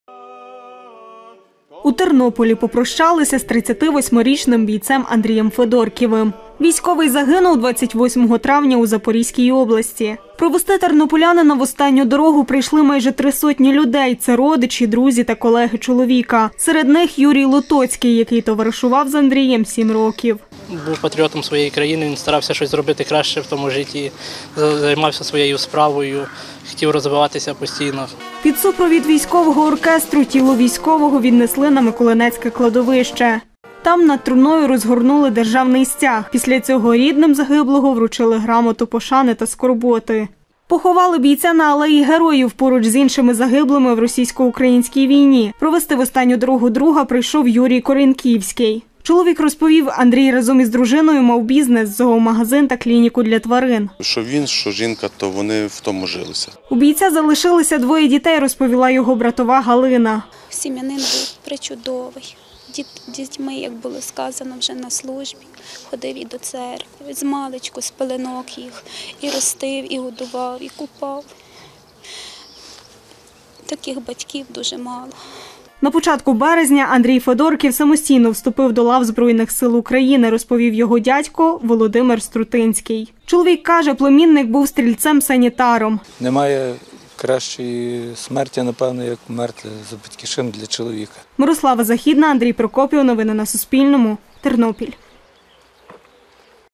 У Тернополі попрощалися з військовим Андрієм Федорківим
У Тернополі попрощалися з 38-річним бійцем Андрієм Федорківим. Військовий загинув 28 травня у Запорізькій області, – розповів Юрій Лотоцький, який товаришував з воїном 7 років.

За його словами, провести захисника в останню дорогу прийшли майже 3 сотні людей – це родичі, друзі та колеги чоловіка.